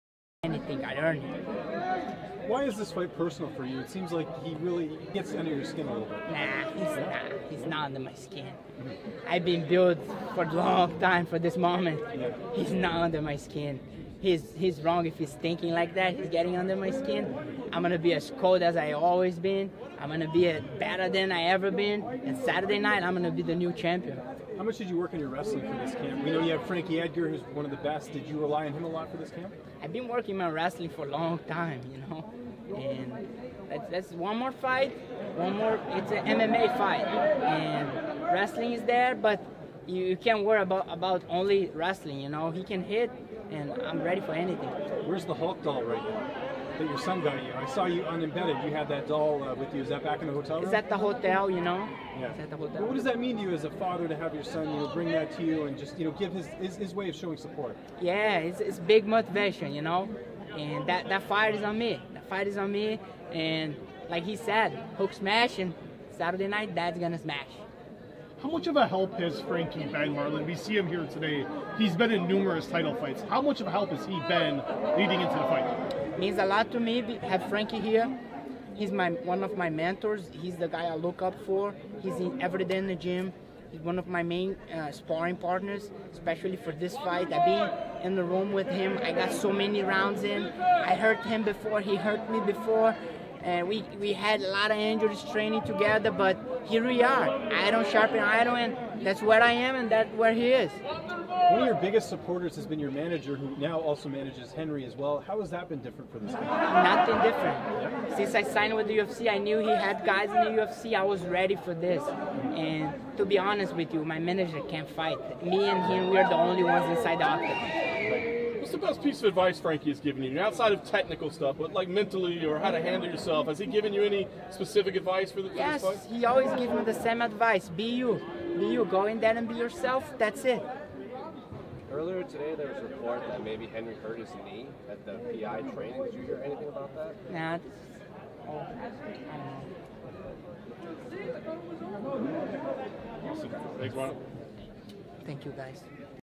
Marlon Moraes UFC 238 Pre-Fight Scrum Video
Marlon Moraes talks with the press in advance of UFC 238.